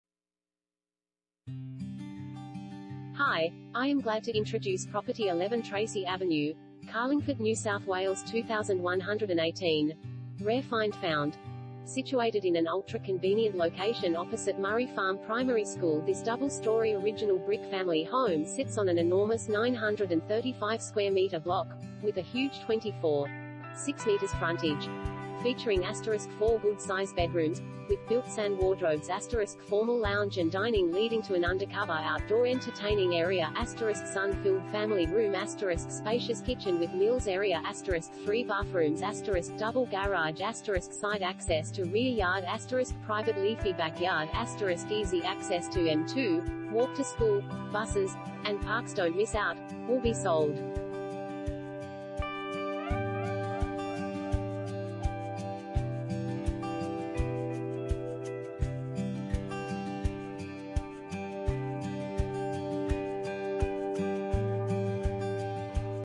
11 Tracey Avenue, Carlingford NSW 2118 | AuPropertyReport.Com
11 Tracey Avenue, Carlingford NSW 2118
Property Type: house
Bedrooms: 4
Bathrooms: 4
Car Space: 2
RARE FIND FOUND